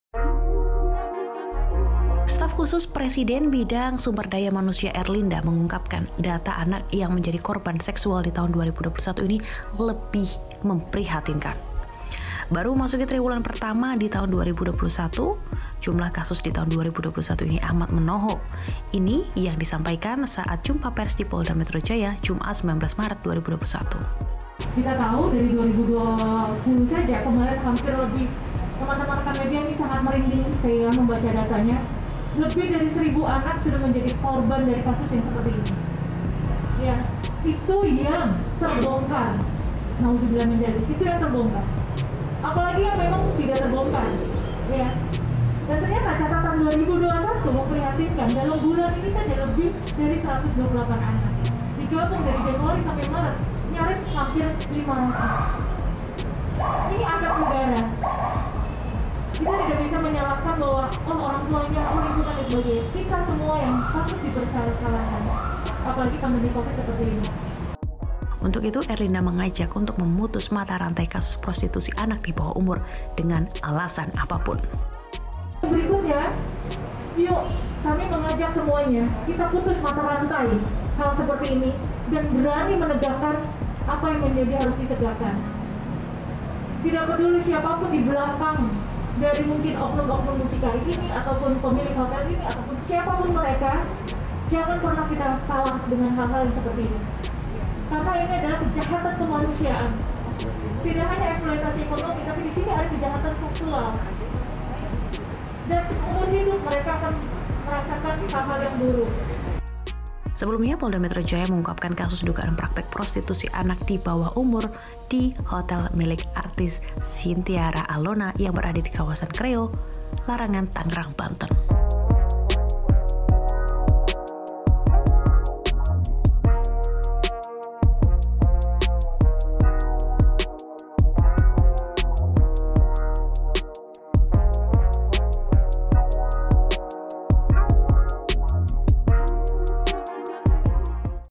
Baru Maret 2021, Jumlah Kasus Prostitusi di Indonesia Bikin Geleng Kepala
Cynthiara Alona ditahan oleh Polda Metro Jaya. Cynthiara Alona ditetapkan sebagai tersangka karena telah menyediakan tempat prostitusi. Polisi kini membongkar motif Cynthiara Alona yang membuka hotelnya jadi tempat ehem-ehem.